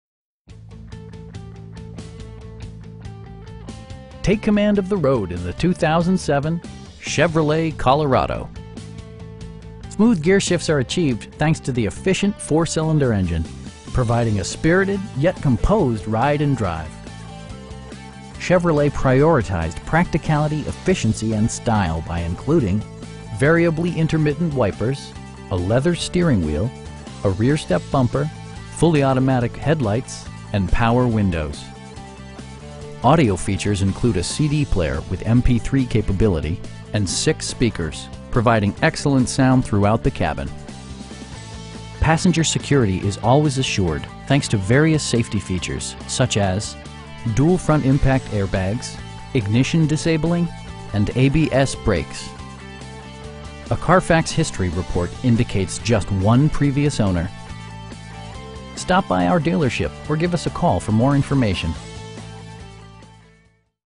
2007 Chevrolet Colorado LT in Tullahoma, TN 37388
Stan McNabb Chevrolet Cadillac Buick GMC

Take command of the road in the 2007 Chevrolet Colorado. Smooth gearshifts are achieved thanks to the efficient 4 cylinder engine, providing a spirited, yet composed ride and drive. Chevrolet prioritized practicality, efficiency, and style by including: variably intermittent wipers, a leather steering wheel, a rear step bumper, fully automatic headlights, power door mirrors, cruise control, and power windows. Audio features include a CD player with MP3 capability, and 6 speakers, providing excellent sound throughout the cabin. Passenger security is always assured thanks to various safety features, such as: dual front impact airbags, ignition disabling, and ABS brakes. A Carfax history report indicates just one previous owner. We offer competitive pricing, and the area's best shopping experience. Stop by our dealership or give us a call for more information.